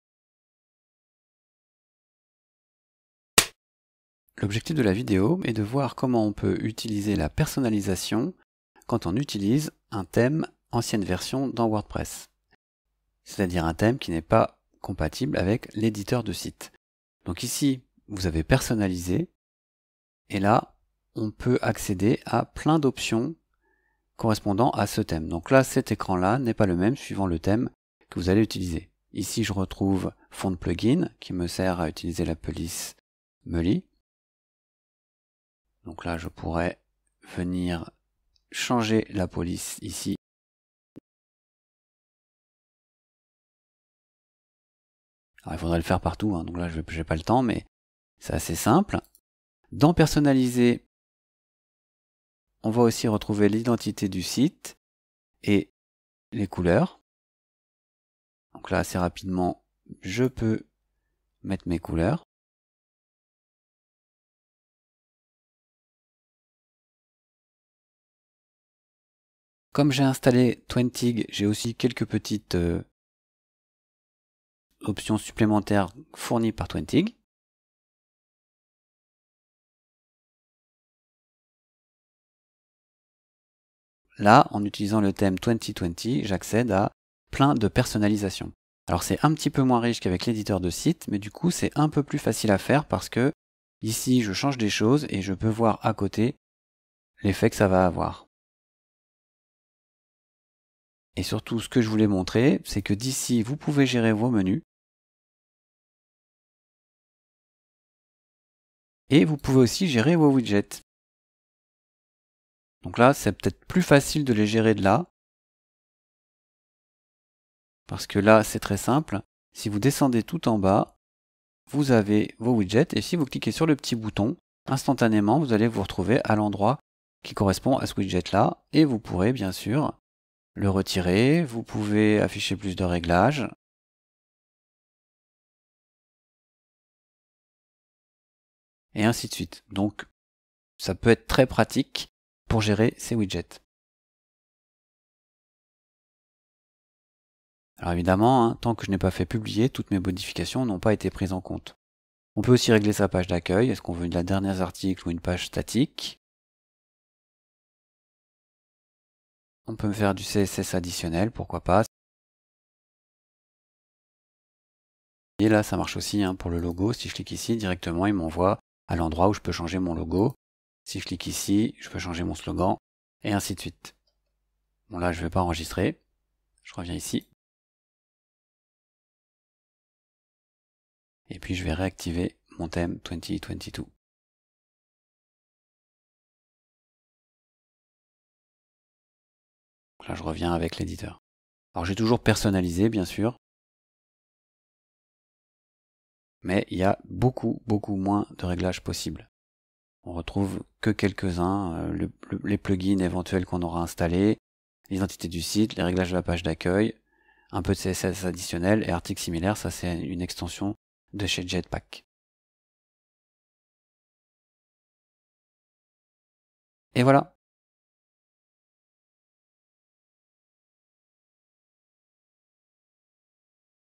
Tuto vidéo : Utiliser l'outil de personnalisation dans WordPress 2022
Points à retenir :
- Apparence - Personnaliser
- Gérer les widgets
- Gérer les menus
- Gérer les couleurs
- Gérer les polices